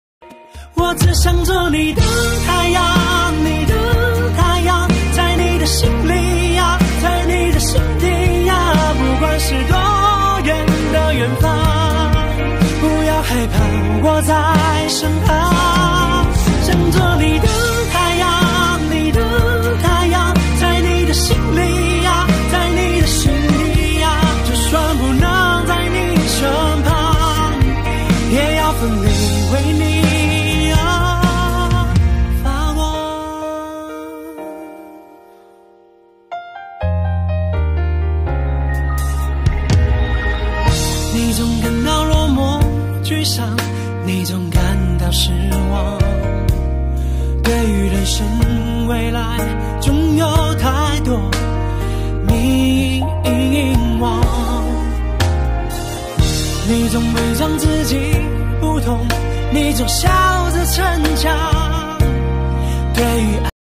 全自动饺子机可以自由更换模具制作水饺 锅贴 咖喱饺 菜饺等 用途非常广泛
阿里巴巴：